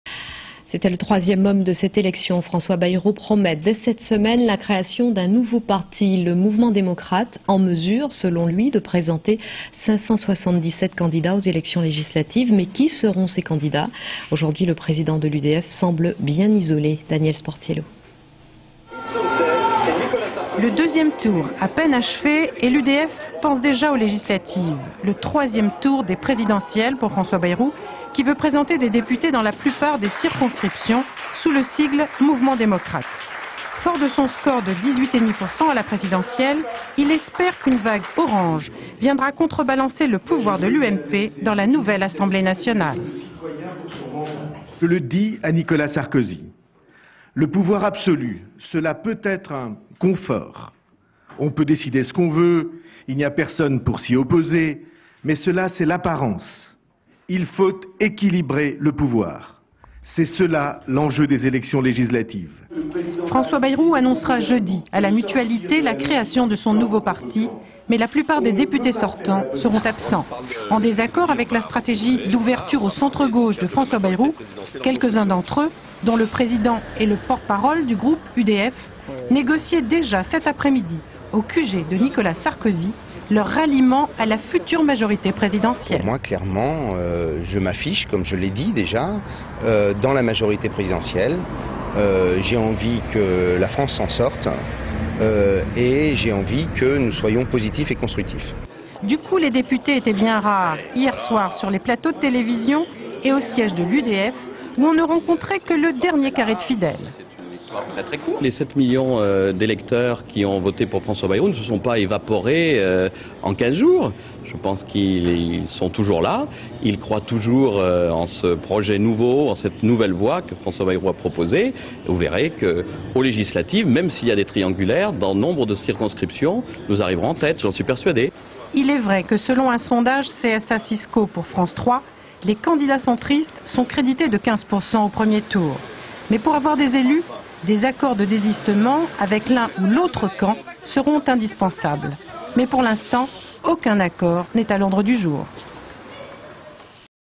Positionnement UDF
7 mai 2007
 Comme les autres partis politiques, l'UDF pense déjà aux prochaines élections législatives et prépare sa stratégie politique. François BAYROU  annoncera prochainement la création d'un nouveau parti politique, le "Mouvement démocrate", et projette de proposer 117 candidats aux élections législatives. Le problème est que la plupart des députés ont déjà rejoint l'UMP et le futur gouvernement. Commentaire sur images factuelles et d'archives, alternant avec un extrait de la déclaration de François BAYROU, président de l'UDF, Hervé MORIN, député de l'Eure et Gilles ARTIGUES, député de la Loire.     François BAYROU doit annoncer cette semaine la creation d'un nouveau parti moins ancré à droite  " mouvement democrate" en mesure de presenter 117 candidats aux législatives      "je le dis à Nicolas SARKOZY, le pouvoir absolu peut être un confort. On peut décider ce qu'on veut, il n'y a personne pour s'y opposer, mais cela c'est l'apparence,Il faut équilibrer le pouvoir. C'est cela l'enjeu des élections législatives..."      Mais problème, la plupart des députés ont rejoint l'UMP dont Hervé MORIN      "Moi très clairement je l'ai dit, je m'affiche dans la majorité présidentielle, j'ai envie que la France s'en sorte et d'être constructif et positif"      Le  dernier carré de fidèles y croit      " les 8 millions d'electeurs qui ont voté Bayrou ne se sont pas évaporés en 15 jours, ils sont là et croient en ce projet nouveau et cette nouvelle voie proposée par BAYROU, vous verrez aux legislatives  malgré des triangulaires nous arriveront en tete" affirme Gilles ARTIGUES       Il faudra cependant des accords de desistement Images d'archive INA